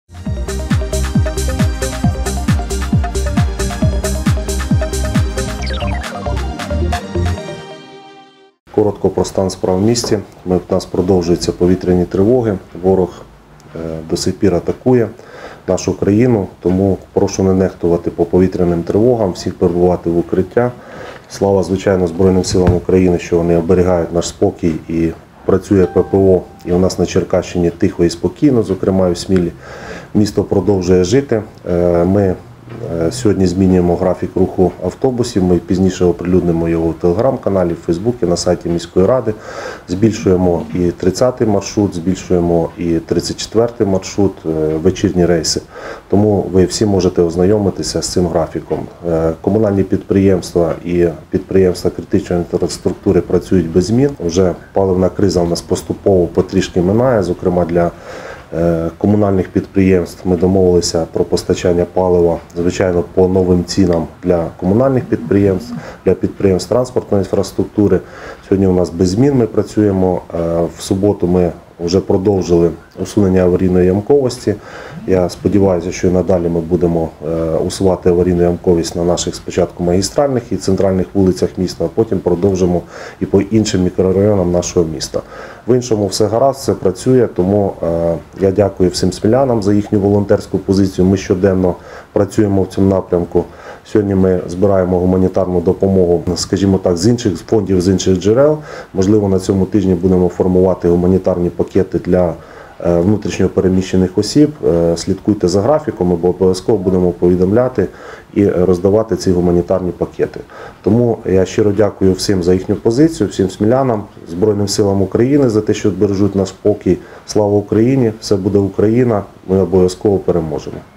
У Смілі збільшились рейси автобусів у вечірній час, готують допомогу для ВПО
00:00 – У Смілі змінюється графік руху автобусів. Деякі маршрути будуть здійснювати рейси довше у вечірній час. Зміни будуть опубліковані найближчим часом
00:48 – Комунальні підприємства міста паливом забезпечені. Продовжується процес усунення аварійної ямковості на дорогах Сміли
01:26 – Цього тижня у Смілі будуть формувати гуманітарні пакети для внутрішньо переміщених осіб. Про графік видачі цієї допомоги буде повідомлено